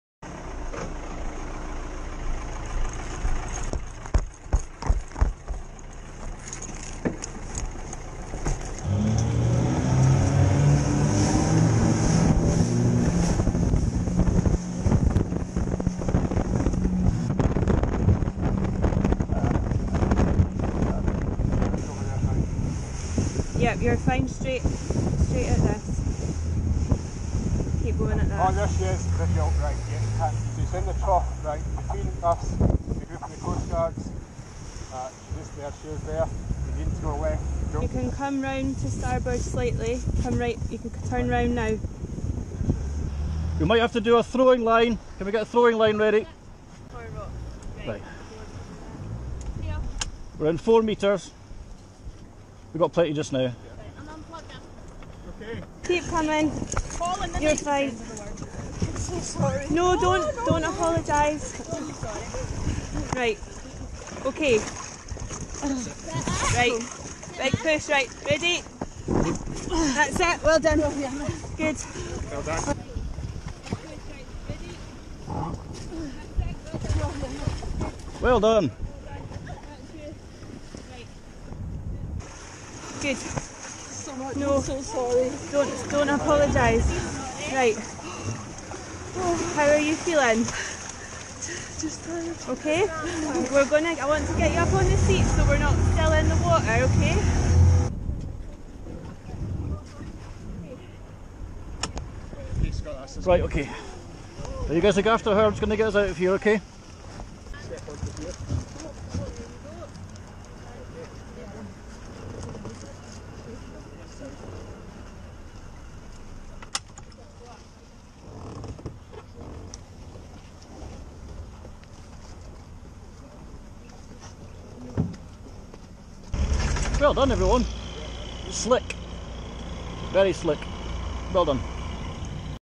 Dramatic footage shows moment swimmer was rescued from rip tide at Kirkcaldy
Credit: Kinghorn/@rnli

A Fife swimmer says she is lucky to be alive after she was rescued from being swept to sea.

A fun dip at Kirkcaldy’s Seafield Beach quickly turned into a nightmare for Karen Eastlake-Bell after a rip tide dragged her to deeper water.

The 53-year-old, a member of Seafield Sinkers open water swimmers group, was knocked off her feet by a large wave on Sunday